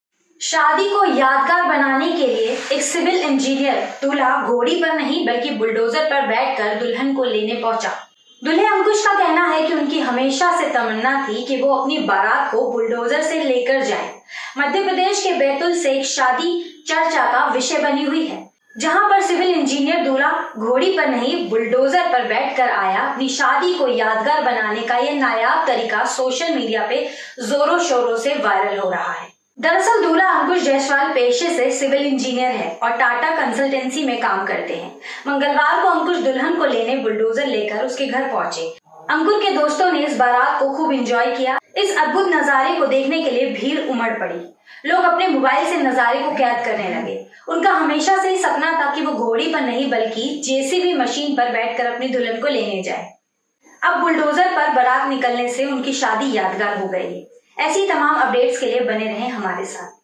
bulldozer से दुल्हन को लेने पहुंचा दूल्हा | Groom On Bulldozer | अनोखी बारात को देखने उमड़ी भीड़
शादी को यादगार बनाने के लिए सिविल इंजीनियर दूल्हा घोड़ी पर नहीं बुलडोजर पर बैठकर दुल्हन को लेने पहुंचा. दूल्हे अंकुश का कहना है कि उनकी हमेशा से तमन्ना थी कि वो अपनी बारात को बुलडोजर से लेकर जाएं. मध्य प्रदेश के बैतूल में एक शादी चर्चा का विषय बनी हुई है. जहां पर सिविल इंजीनियर दूल्हा घोड़ी पर नहीं बुलडोजर पर बैठकर आया. दरअसल दूल्हा अंकुश जायसवाल पेशे से सिविल इंजीनियर है और टाटा कंसल्टेंसी में काम करते हैं